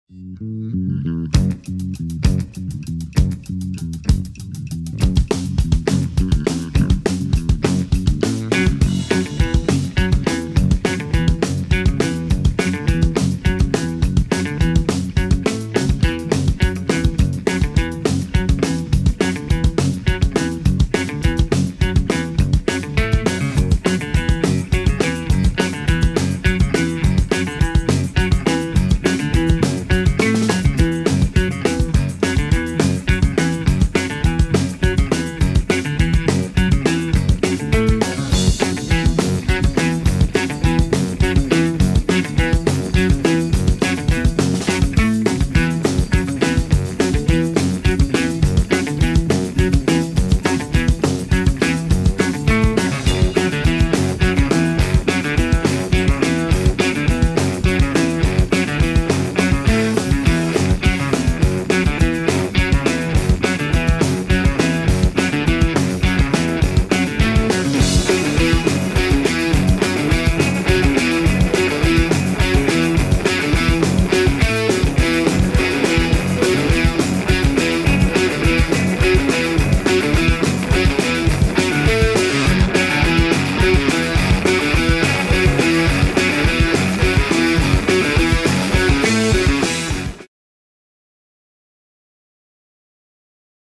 V8 Interceptor - quick illustration ink and colour
A quick illustration  of a V8 Interceptor - I rarely draw cars so I thought I'd better practice. It's ok.
However if you look closely the door isn't right at all-  notice the exhaust pipes would prevent the door from opening.
I'll be posting a bunch of these quick sketches let me know if you like them.
Tunes by: Captain Kickarse and the Awesomes - find them here